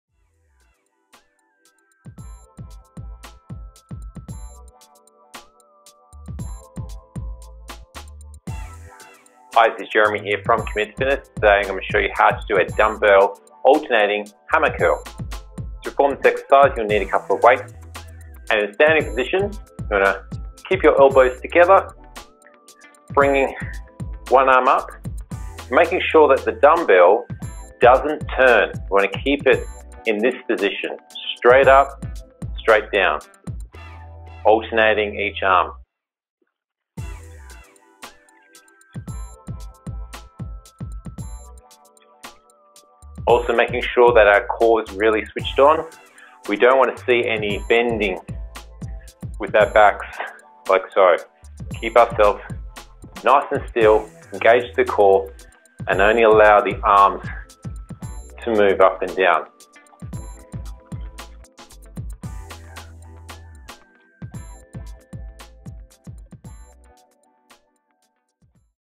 Dumbbell Alternating Hammer Curls
Video demonstrating the proper way to perform a dumbbell alternating dumbbell curl.